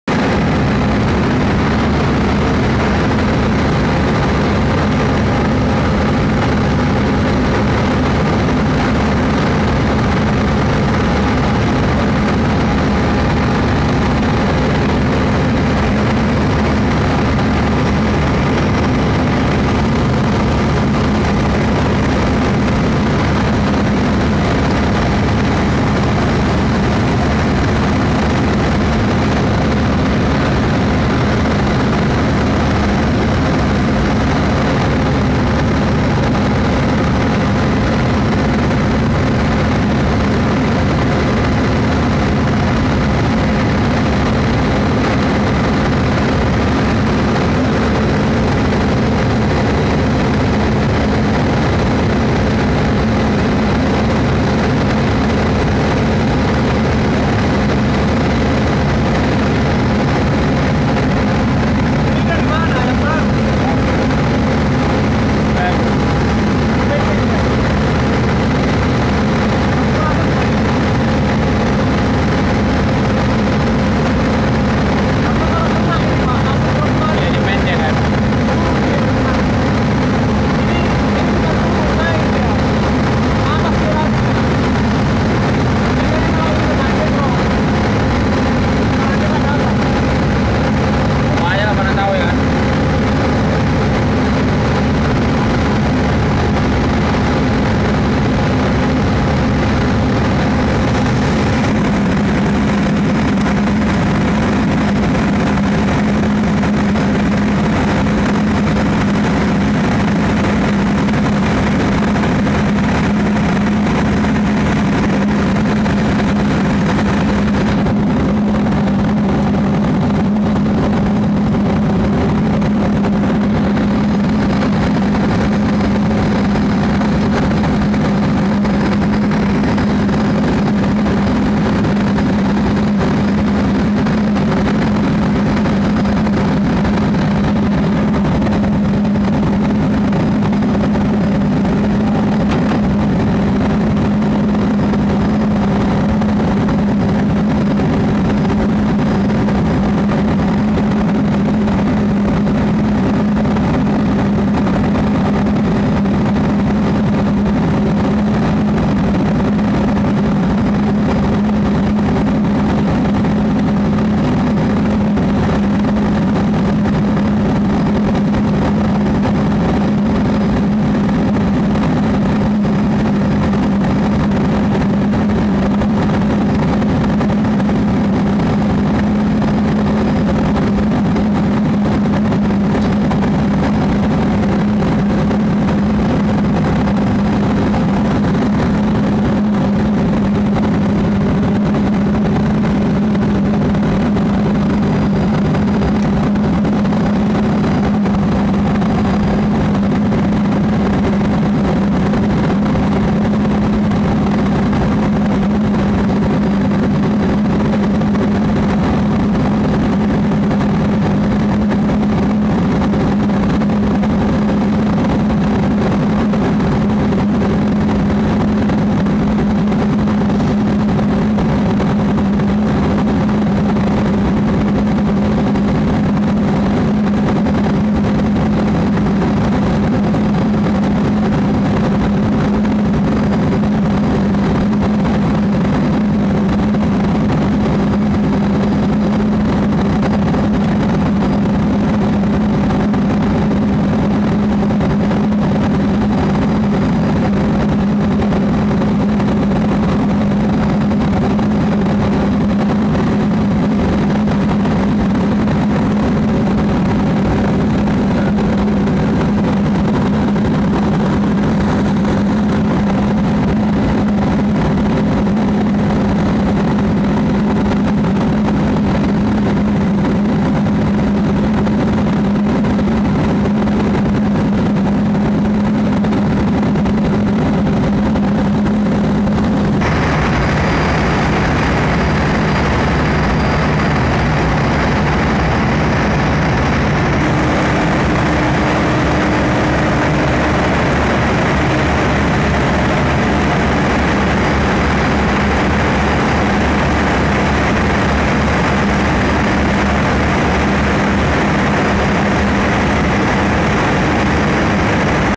cara memperbaiki frekuensi genset yang tidak stabil ( Genset Caterpillar)
berikut adalah tips memperbaiki genset caterpilar yang masalah nya pada frekuensinya tidak stabil atau naik turun